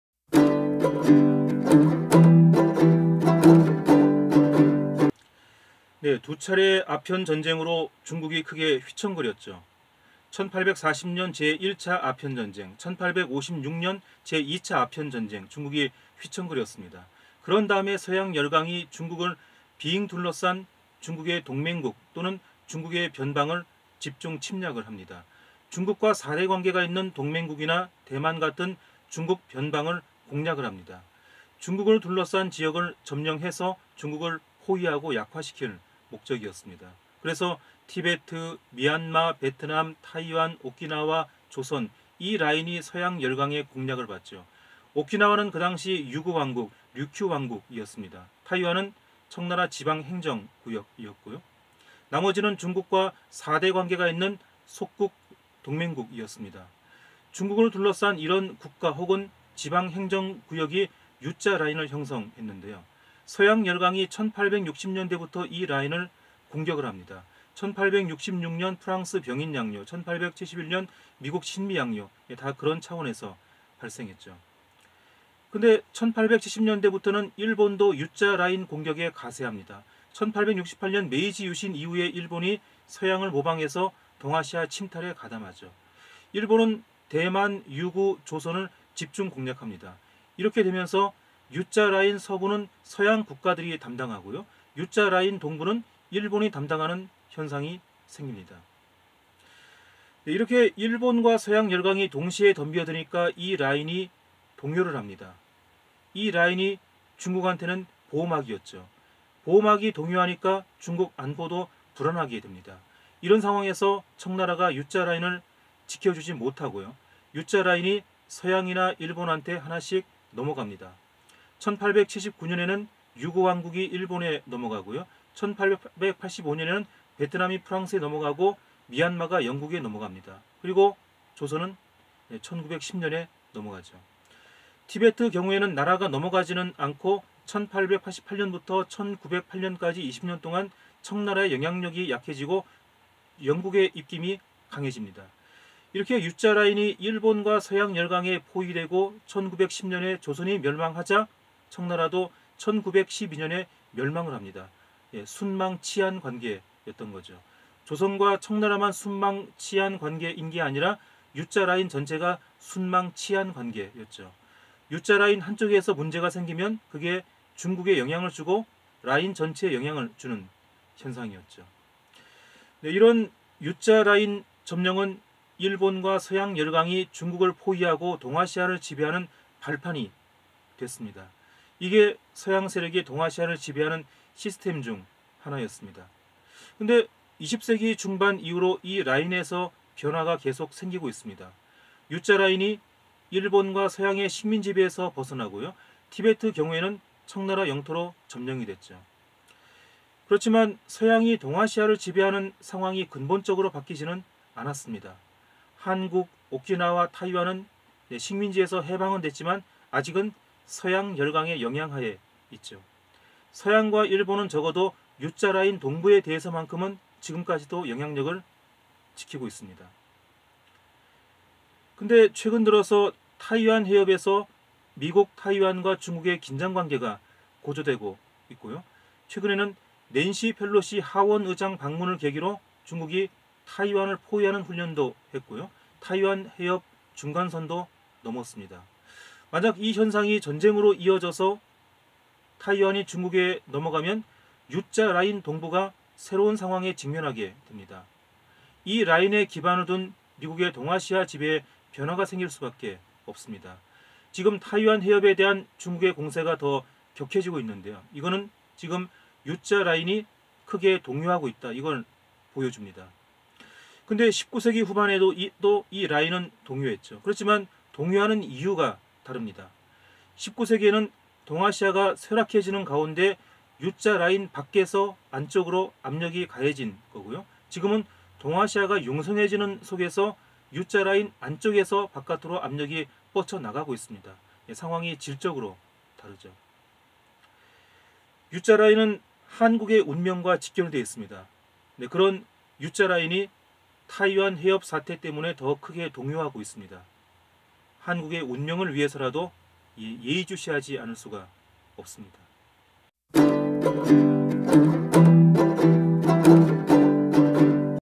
중국 포위 U자 라인이 흔들린다(KOREA 진단)
19세기 중후반부터 중국을 빙 둘러싸는 티베트-미얀마-베트남-대만-오키나와-조선의 U자 라인이 동요하면서 조선이 멸망하고 청나라가 멸망했습니다. 그런데 지금 이 U자 라인이 다시 동요하고 있습나디. 구체적 내용을 역사학자 김종성이 말씀드립니다.